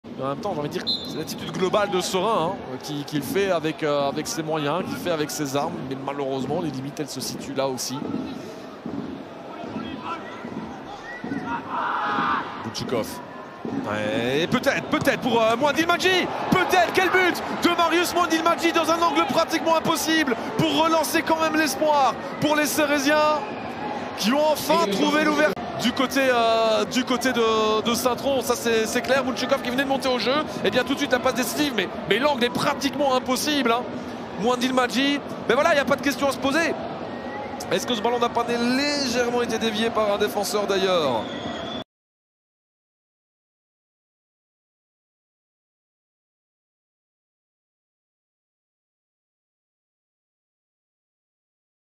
GOAL : Marius MOUANDILMADJI 2-1 | 🟡🔵 STVV vs RFC Seraing 🔴⚫️ | 18.03.2023
⚽ 26ème goal de la saison 2022/2023 - D1A - Pro League.